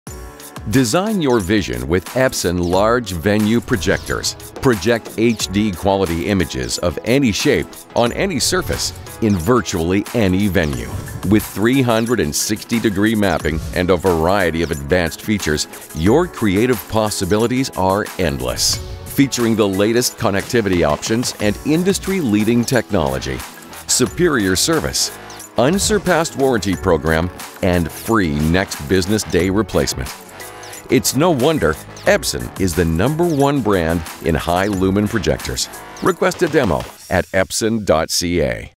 Epson Large Venue Projectors | Take the Tour
Epson PowerLite, Pro G, and Pro Z large-venue projectors offer professional projection without compromise.